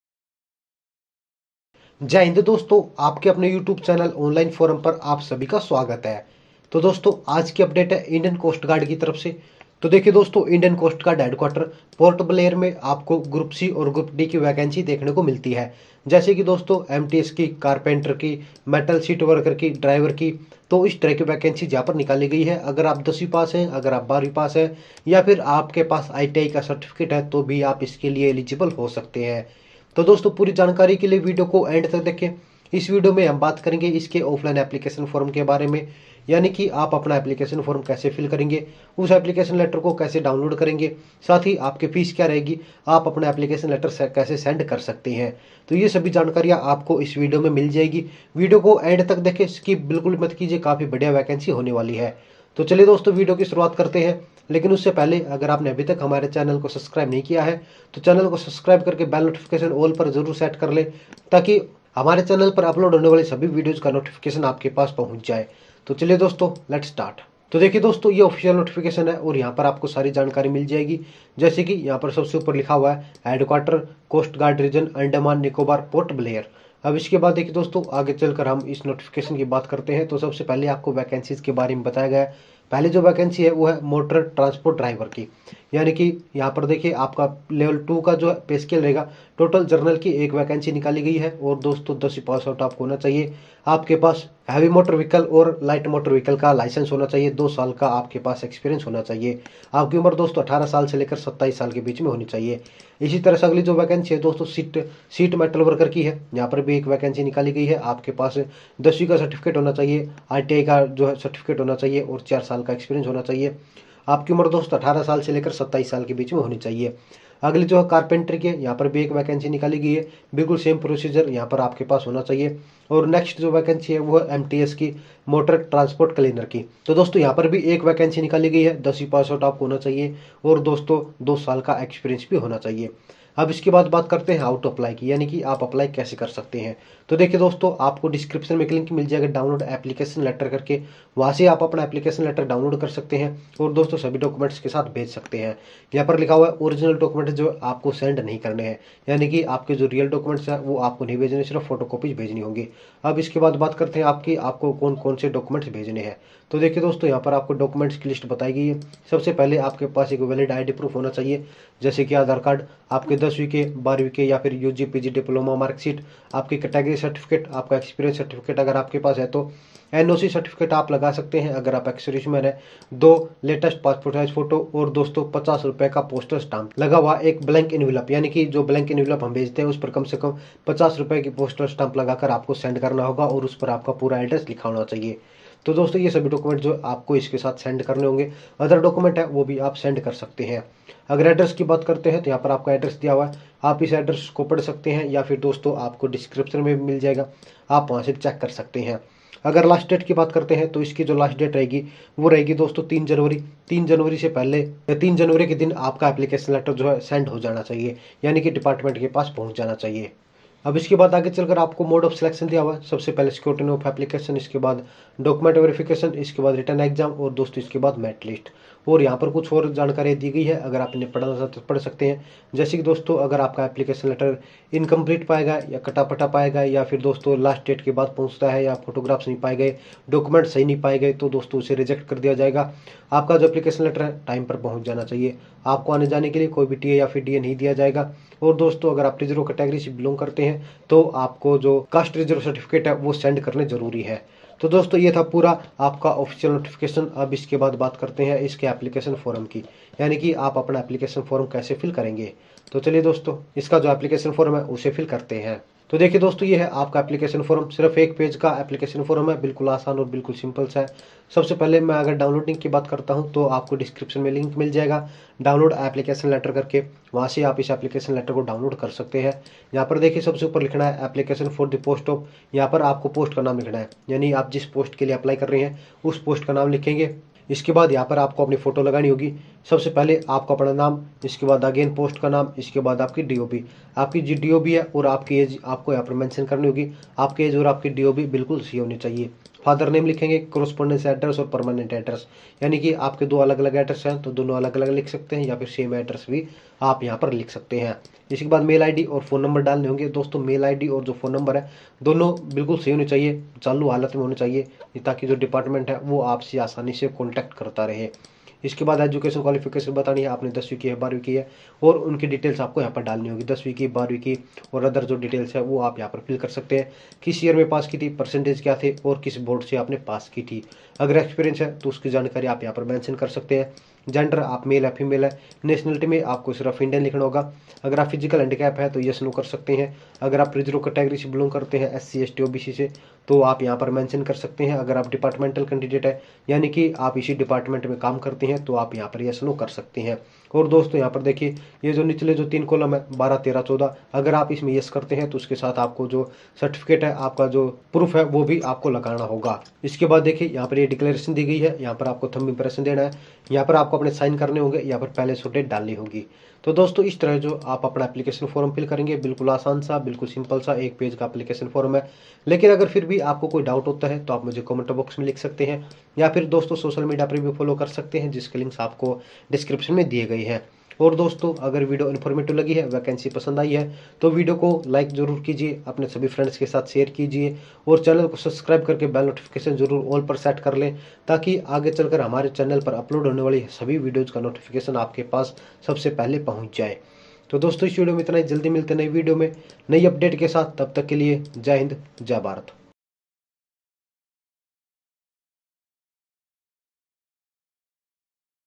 HQ Indian Coast Guard Port Blair Region Group C Offline Form 2022 Kaise Bhare | ICG Offline Vacancy
“Indian Coast Guard Region (A&N), Port Blair” invites applications from candidates who have qualified respective educational qualifications to fill vacancies of Motor Transport Driver (OG), Sheet Metal Worker (Skilled), Carpenter (Skilled). The candidates who want to get this opportunity can apply through offline mode.

Indian Coast Guard Port Blair Recruitment 2022 Offline Application Form:

Last Date: 03 January 2023

Fees: 0/-

Age: 18 to 27 Years

Vacancy: 04

Qualification:
Civilian Motor Transport Driver (Ordinary Grade):
10th standard pass. Must possess valid driving licence for both heavy and light Motor Vehicles.
Should have at least two years experience in driving Motor Vehicles.
Must have Knowledge of motor mechanism.

Carpenter (Skilled):
Should have completed apprenticeship in the trade from Industrial Training Institute or other recognised Institution. OR
Should have complete training course in the relevant trade from an ITI and One year trade experience. OR
Should have 4 years experience in the trade.

MTS:
10th standard pass. Two years experience in mechnical workkshop.
For more details read the official notification.

Postal Address: “The Commander, Coast Guard Region (A&N), Post Box No. 716, Haddo (PO), Port Blair 744 102, A&N Islands”

Candidates should reach their application form along with self attested copies of necessary documents at the following address before or on the last date of application By offline mode only. (Ordinary post only)

Our Aim is to Provide the Best Learning Experience. And its Benefit is to be Given Free of Cost to More and More Youth of the Country.

Note: Don't Call On This Number.